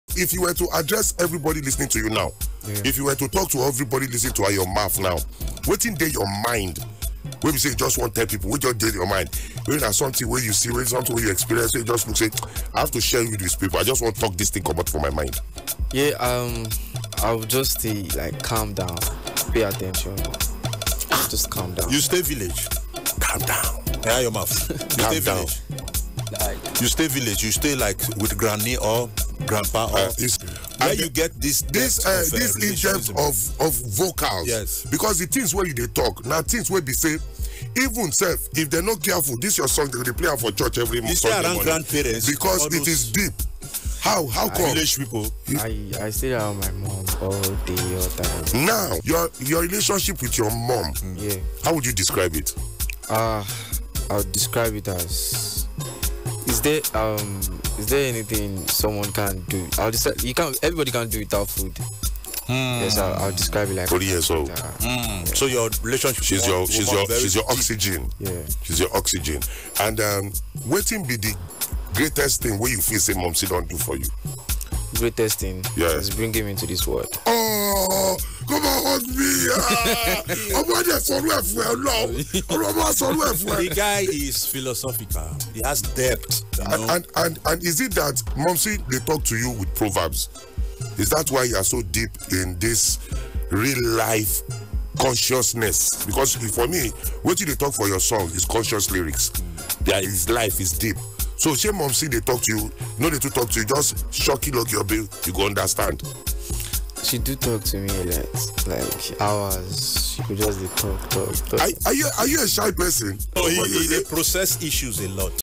"The Relationship with My Mom Made Me Who I Am Today" – Ayo Maff Shares His Experience Growing Up
Ayo Maff reflects on how his strong bond with his mother shaped his life and career.

For more entertainment gist, the latest songs, and everything entertainment, join the Max Family on social media -